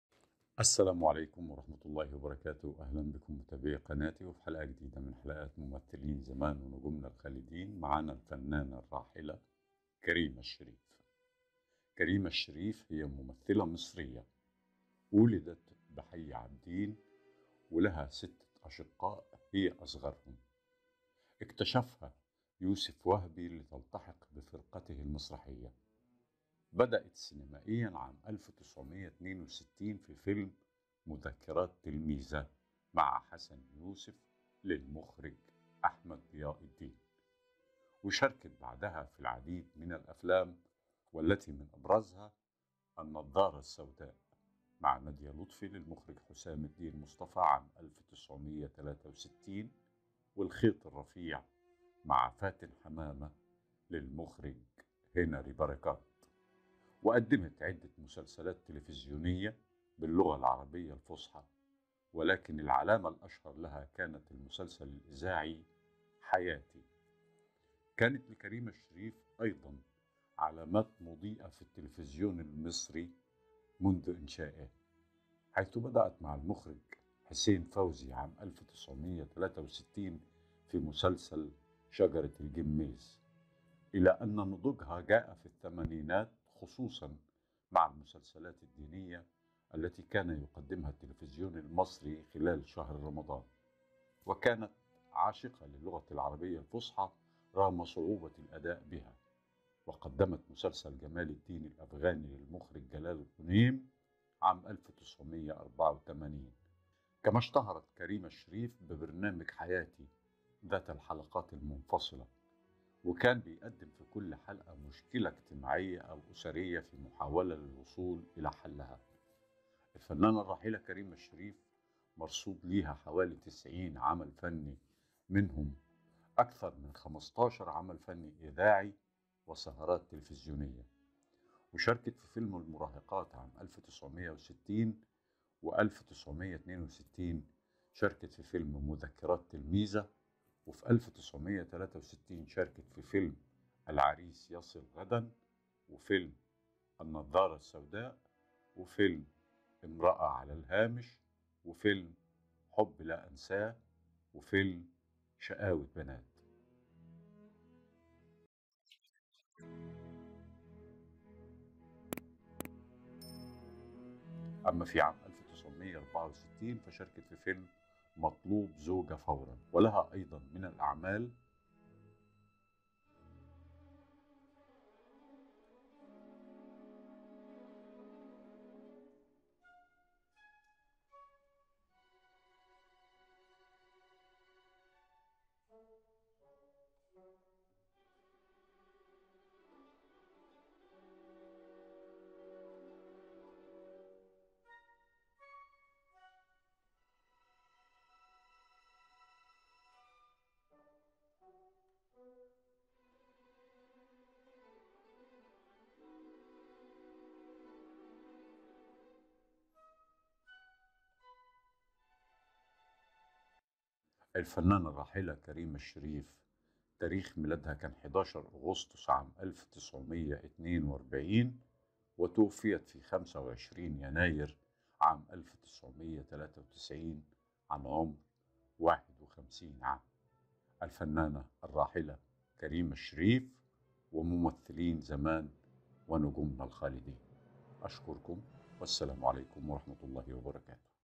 كريمه الشريف - ضحكتها غابت مبكرا
الممثلة الراحلة / كريمة الشريف التى توفيت عن عمر 51 عاما

تاريخ رفع الفيديو غير مدرج : 26/01/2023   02:23am
تاريـــخ نشر الفيديو علنـــى  : 23/02/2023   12:00am